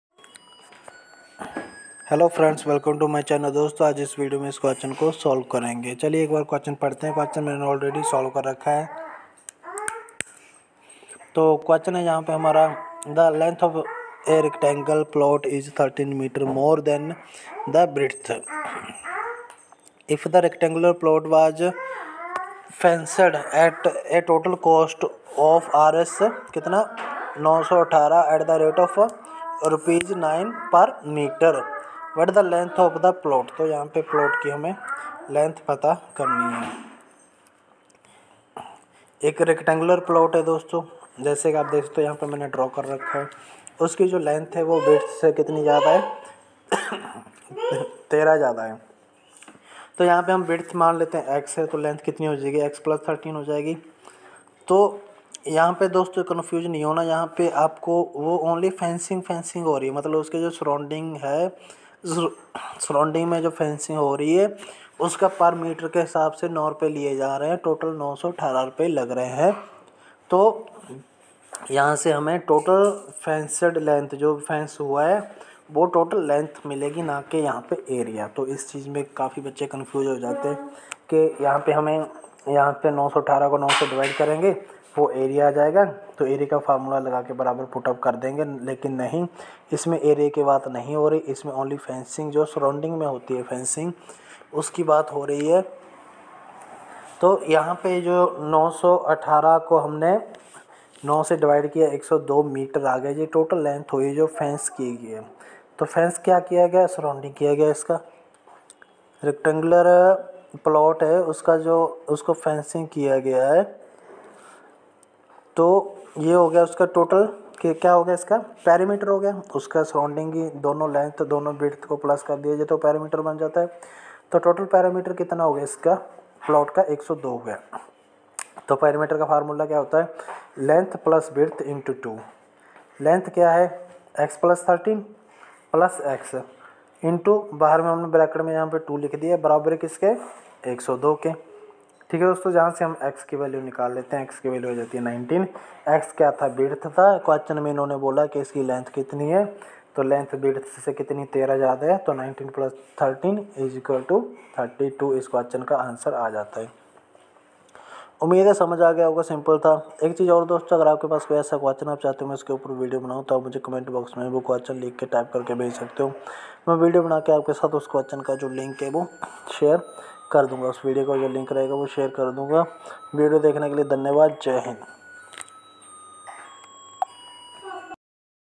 The length of a rectangular plot is 13 metres more than its breadth. If the rectangular plot was
The length of a rectangular plot is 13 metres more than its breadth., If the rectangular plot was fenced at a total cost of Rs 918 at the rate of Rs 9 per metre, what is the length of the plot? (in metres)